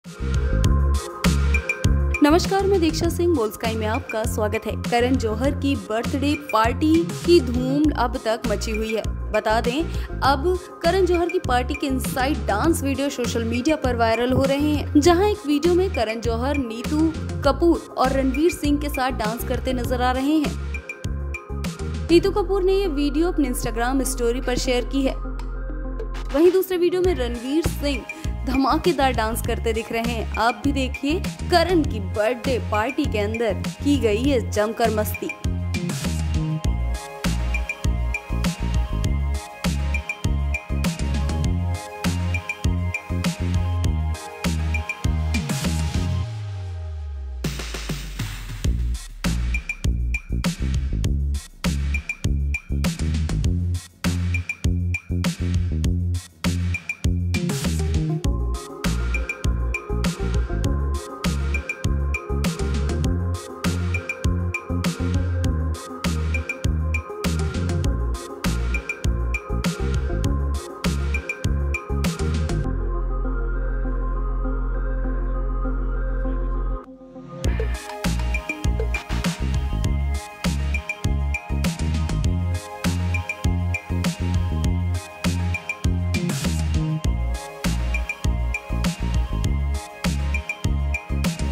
Karan Johar Birthday Party Ranveer Singh Neetu Kapoor Inside Dance Video Viral | Boldsky
Videos and photos of Karan Johar's birthday party are becoming fiercely viral on social media. Let us tell you that now the dance video of Ranveer Singh and Neetu Kapoor from Karan Johar's birthday party is becoming fiercely viral.

करण जौहर की बर्थ डे पार्टी की वीडियोज और फोटोज सोशल मीडिया पर जमकर वायरल हो रही है। बता दें कि अब करण जौहर की बर्थ डे पार्टी से रणवीर सिंह और नीतू कपूर का डांस वीडियो जमकर वायरल हो रहा है।